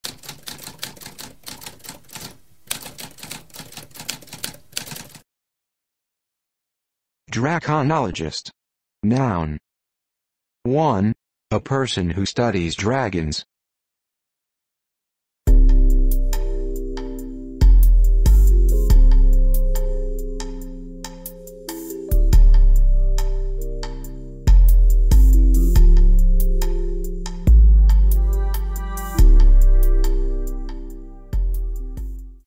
What does draconologist mean?
A spoken definition of draconologist.

Intro Sound:
Typewriter - Tamskp